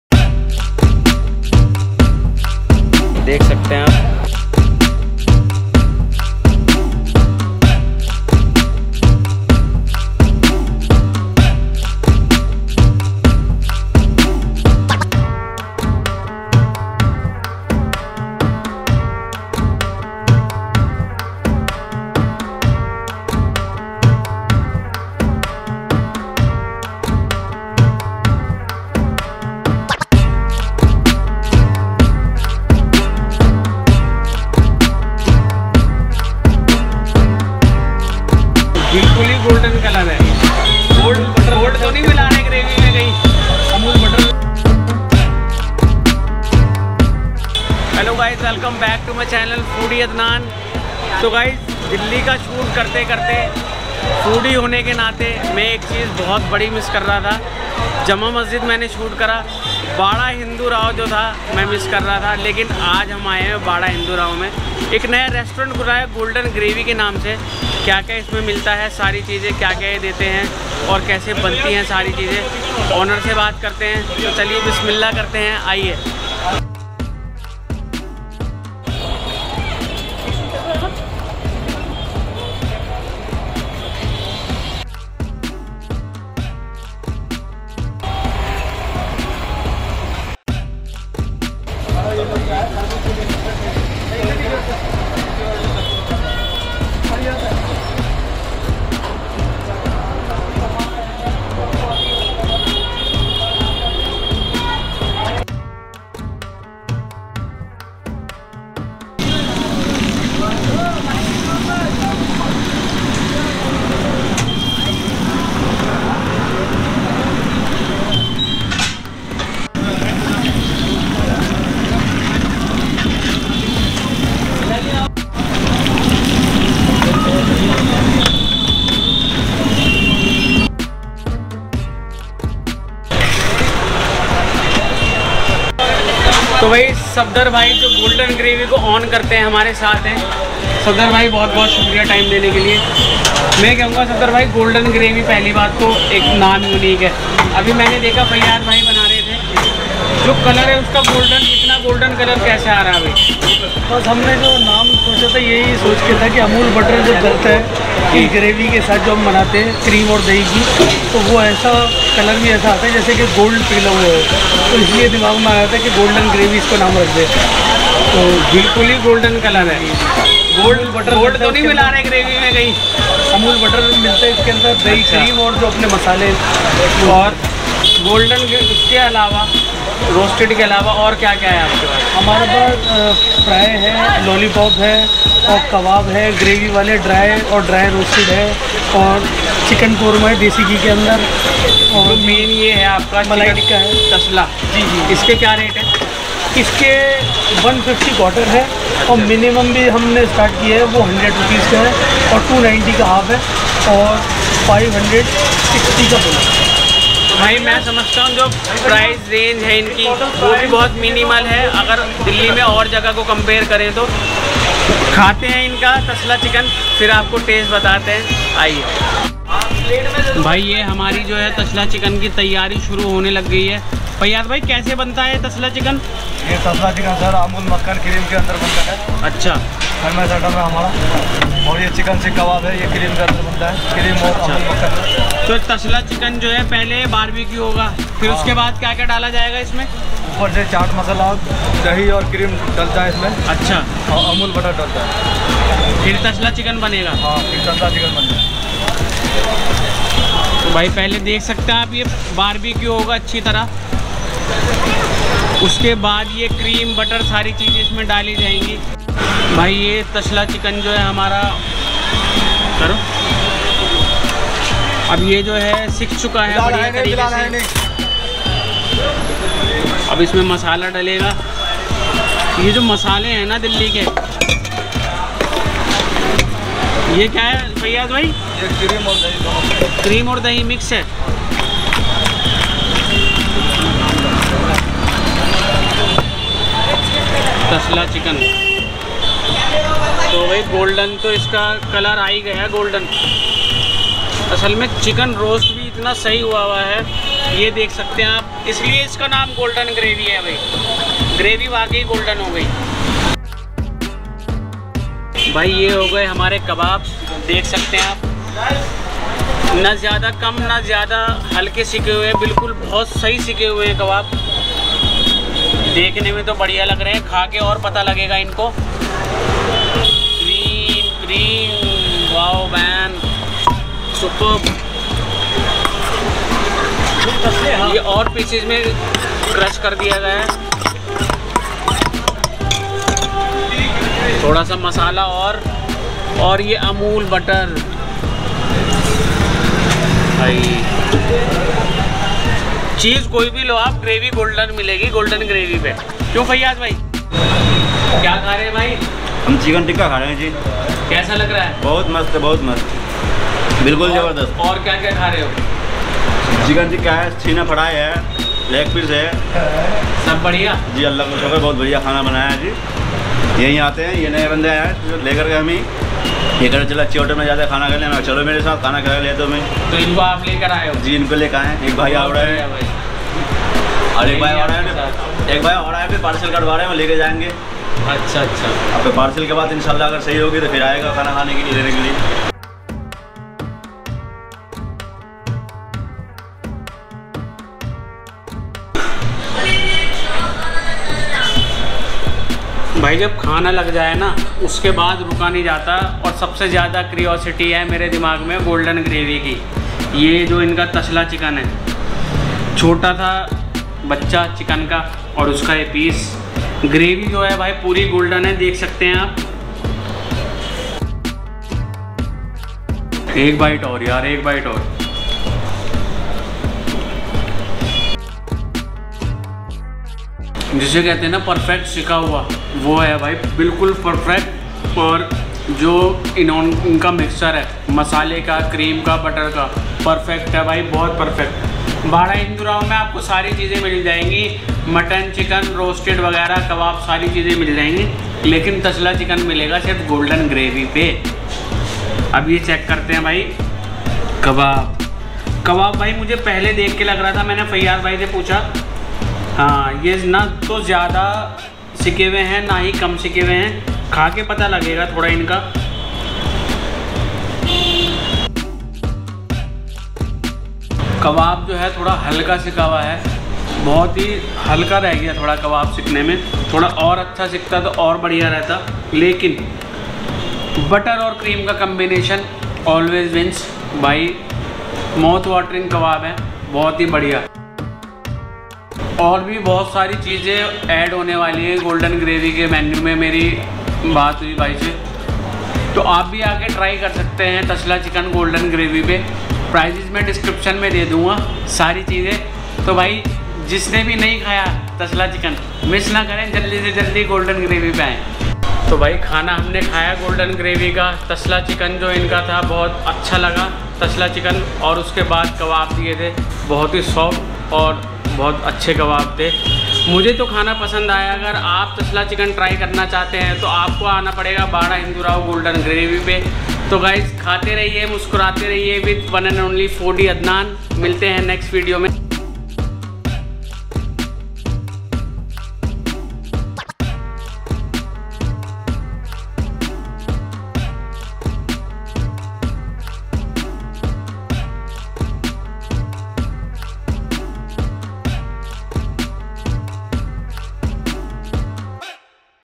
SPECIAL TASLA CHICKEN OF BARA HINDU RAO || BEST ROASTED CHICKEN IN DELHI || GOLDEN GRAVY
Hey Everyone... Hope you all are doing well and enjoying my videos. I'm loving exploring better taste for you guys. Golden Gravy entered in food market with their special items known as "TASLA CHICKEN". They Launched it after a lot of practice and research. The new restaurant GOLDEN GRAVY known for its combination of cream and dahi with mixture of amul butter which turns it into golden colour and that makes it unique from others.
They also serve Chicken malai Tikka, Chicken Korma and Chicken Changezi as well. Golden Gravy the name I found interesting.

I tried  Tasla Chicken and Kabab. Both are rich in tatse and as well as quality.
And of course their foremost the GOLDEN GRAVY was really surprising. Their combination of spices, cream and butter was perfect. I can bet on you that you will also love this. So.. Go & Try Soon.

Special Thanks To SAFDAR BHAI (Owner at GOLDEN GRAVY) for giving  his valuable time.

Address : - Shop no - 8020 Main Road Bara Hindu Rao Near Masjid SHAIKHAN, DELHI -110006

KEEP SUPPORTING |KEEP LOVING |KEEP EATING WITH FOODIE ADNAN

CREDITS

VIDEO :- LAKSH FILMS